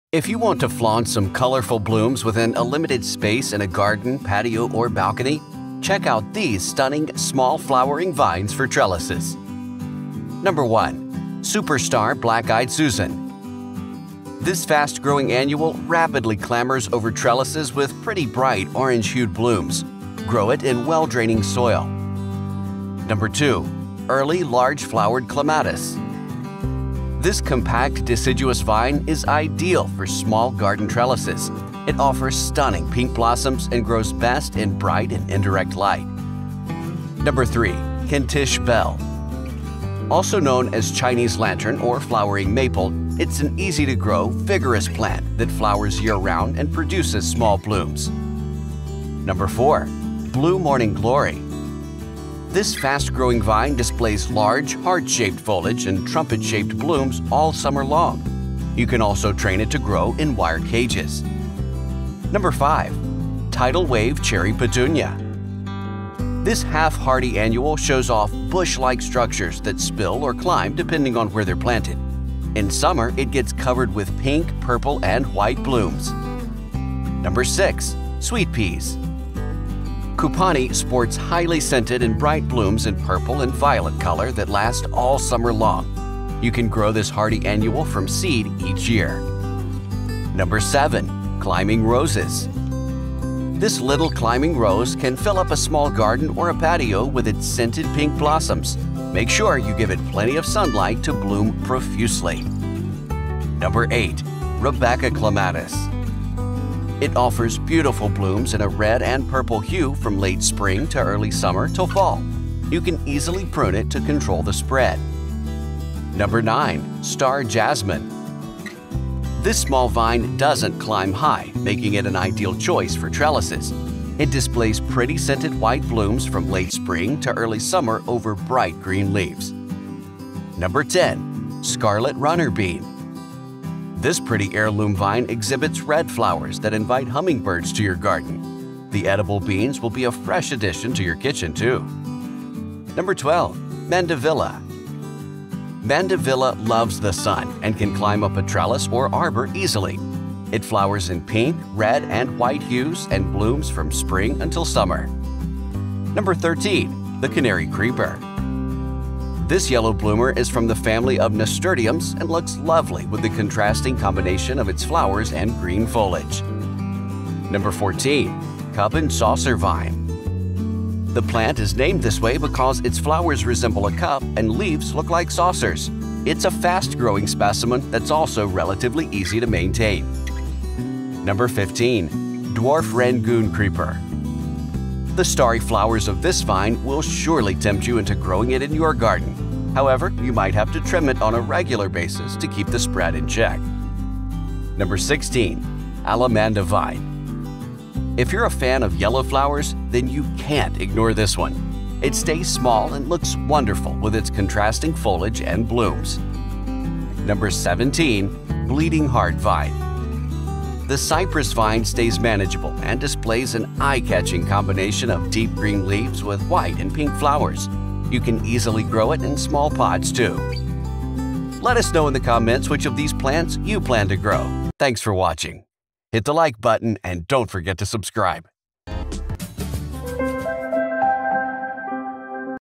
17 Small Flowering Vines For Trellises
If you want to flaunt some colorful blooms within a limited space in a garden, patio, or balcony, check out these stunning Small Flowering Vines for Trellises!

Small Flowering Vines for Trellises
The plants on this list might require pruning from time to time if you want to confine them to a space of your liking.
1. Superstar Black-Eyed Susan

This fast-growing annual rapidly clambers over trellises with pretty bright orange-hued blooms. Grow it in well-draining soil.
2. Early Large-Flowered Clematis

This compact deciduous vine is ideal for small garden trellises. It offers stunning pink/lilac blossoms and grows best in bright and indirect light.
3. Kentish Belle

Also known as ‘Chinese Lantern’ or ‘Flowering Maple’, it is an easy-to-grow, vigorous plant that flowers year-round and produces small blooms.
4. Blue Morning Glory

This fast-growing vine displays large, heart-shaped foliage and trumpet-shaped blooms all summer long. You can also train it to grow in wire cages.
5. Tidal Wave Cherry Petunia

This half-hardy annual shows off bush-like structures that spill or climb depending on where they are planted. In summer, it gets covered with pink, purple, and white blooms.

6. Sweet Peas

‘Cupani’ sports highly scented and bright blooms in purple and violet color that last all summer long. You can grow this hardy annual from seed each year.
7. Climbing Roses

This little climbing rose can fill up a small garden or a patio with its scented pink blossoms. Make sure you give it plenty of sunlight to bloom profusely.
8. Rebecca Clematis

It offers beautiful blooms in a red and purple hue from late spring to early summer till fall. You can easily prune it to control the spread.

9. Star Jasmine

This small vine doesn’t climb high, making it an ideal choice for trellises. It displays pretty scented white blooms from late spring to early summer over bright green leaves.
10. Scarlet Runner Bean

This pretty heirloom vine exhibits red flowers that invite hummingbirds to your garden. The edible beans will be a fresh addition to your kitchen too!
12. Mandevilla

Mandevilla loves the sun and can climb up a trellis or arbor easily. It flowers in pink, red, and white hues and blooms from spring to summer.
13. The Canary Creeper

This yellow bloomer is from the family of nasturtiums and looks lovely with the contrasting combination of its flowers and green foliage.
14. Cup and Saucer Vine

The plant is named this way because its flowers resemble a cup, and leaves look like saucers. It is a fast-growing specimen that is also relatively easy to maintain.
15. Dwarf Rangoon Creeper

The starry flowers of this vine will surely tempt you into growing it in your garden. However, you might have to trim it on a regular basis to keep the spread in check.
16. Allamanda Vine

If you are a fan of yellow flowers, then you can’t ignore this one! It stays small and looks wonderful with its contrasting foliage and blooms!
17. Bleeding Heart Vine

The cypress vine stays manageable and displays an eye-catching combination of deep green leaves with white and pink flowers. You can easily grow it in small pots too.